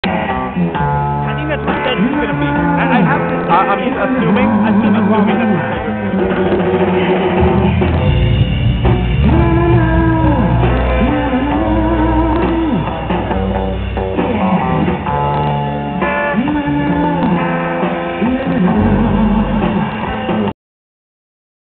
The Mars Volta - preshowmusic
just hangin' around, waitin' for TMV to come on.